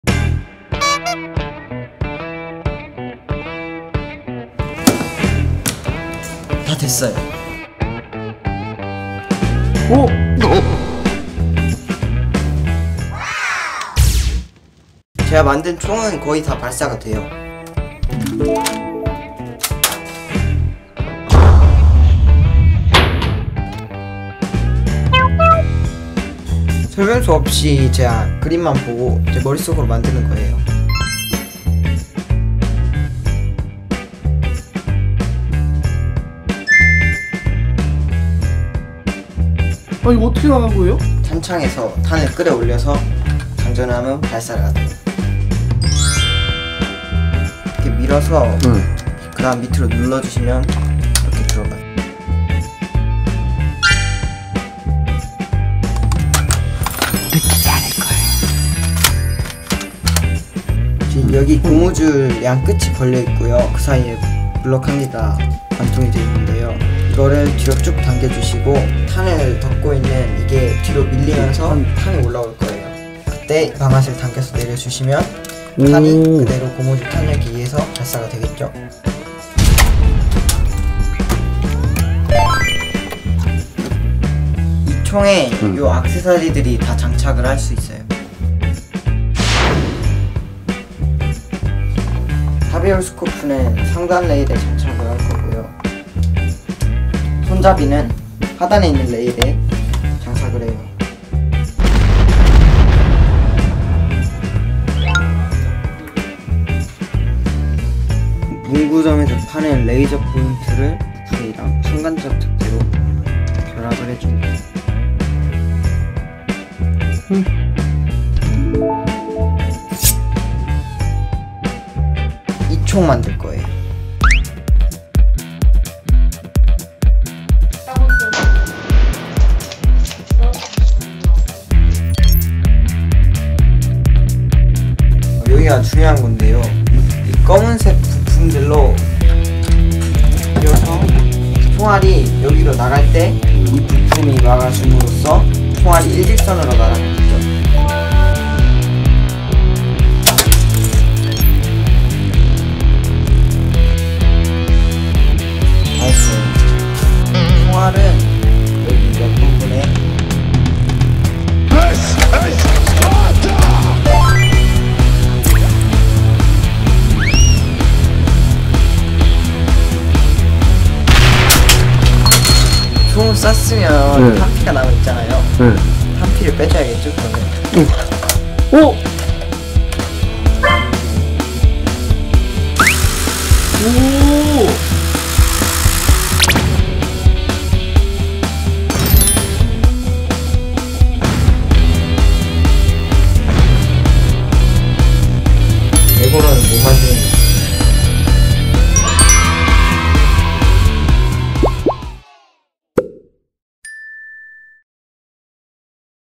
총 만드는 13살 초딩 (feat.레고) │13-Year-Old gun maker (feat. LEGO)
만든 총이 발사까지 된다는거슨... 이건 진짜 총이라고 할 수 있겠죠??ㄷㄷㄷ
레고로 디테일한 무기가 만들어진다니
그것도 초등학생이!! 천재 아님?

▶세상에 이런 동물이?!

▶조금 평범하지 않은 먹방 플레이어(취존영역)